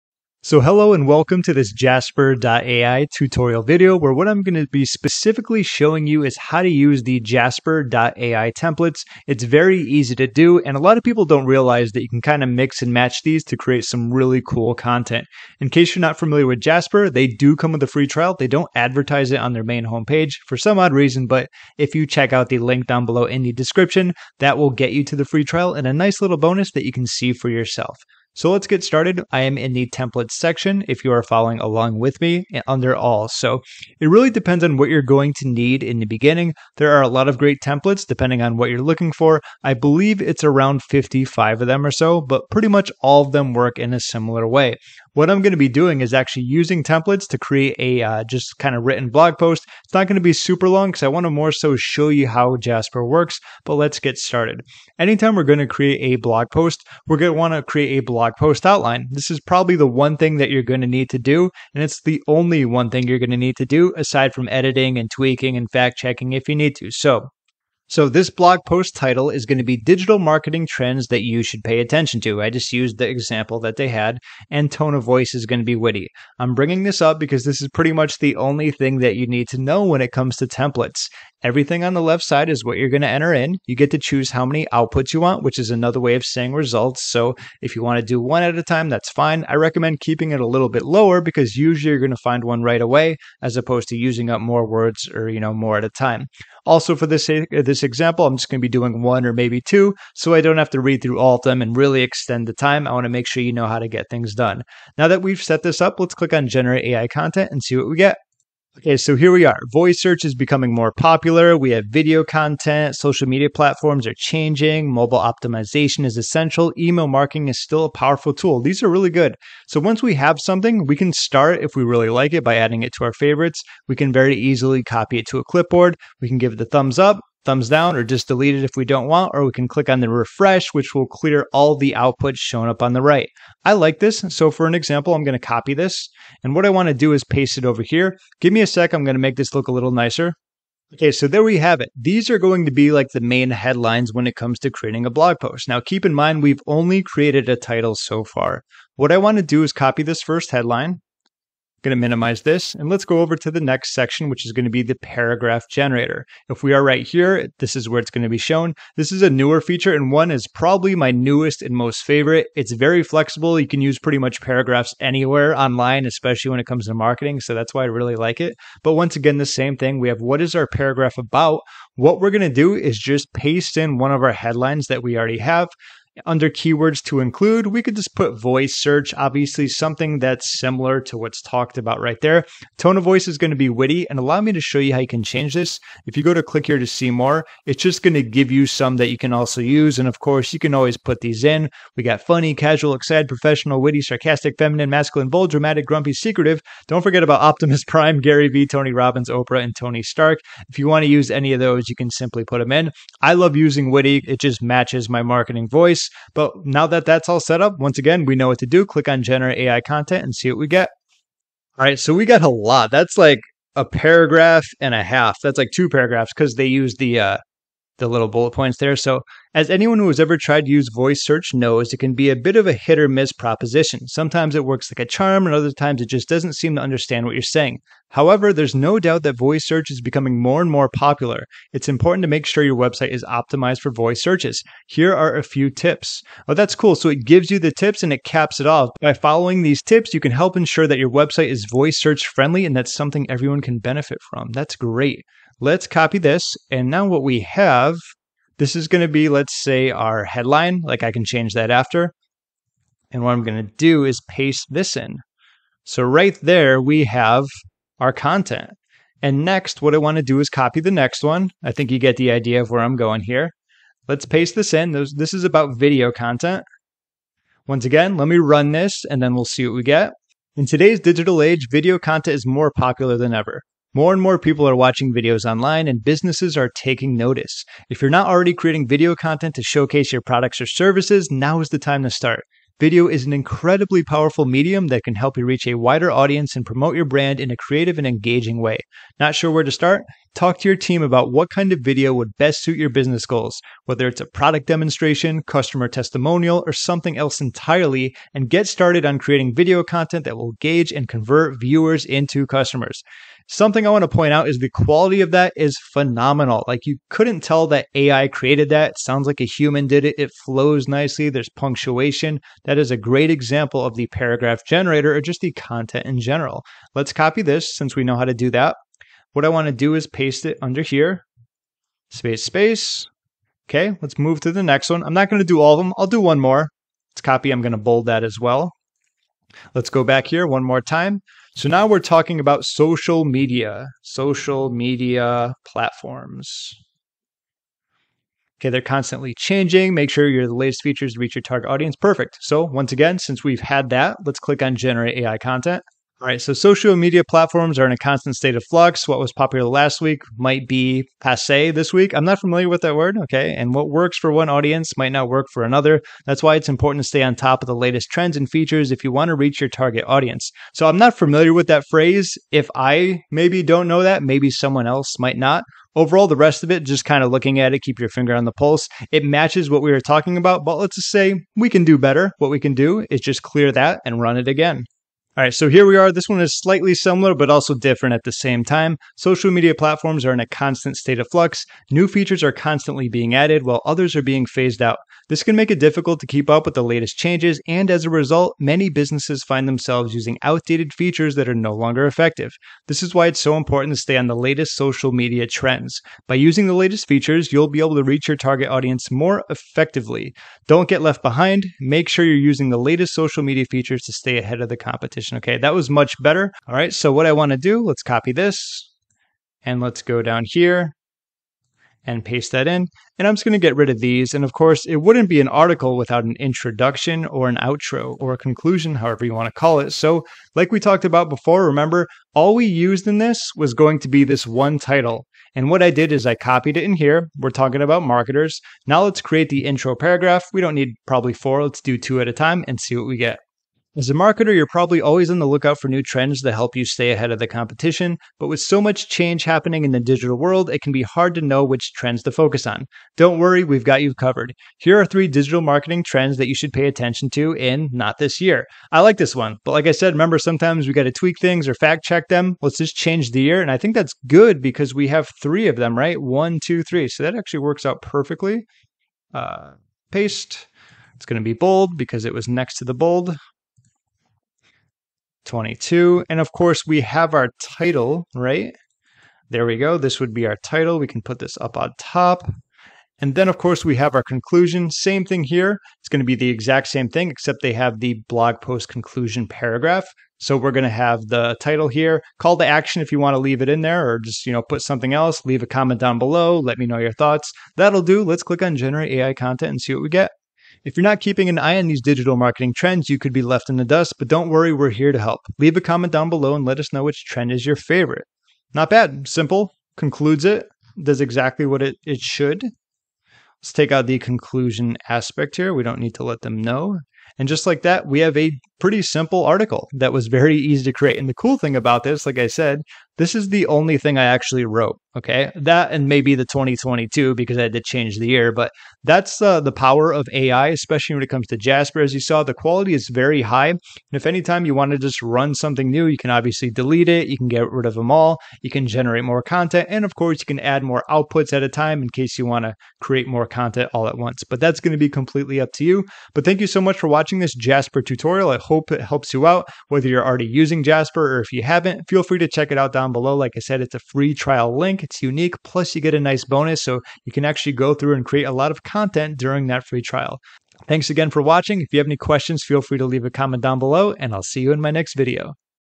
Jasper.ai Tutorial: How To Use Jasper.ai Templates (Examples)
Jasper.ai Tutorial Video

This Jasper.ai tutorial will show you how to quickly and easily use the Jasper.ai templates. Even better, I'll show you how to get creative and turn a few templates into an actual blog post, while only writing a few words myself. Check out the video to see all of the magic in action.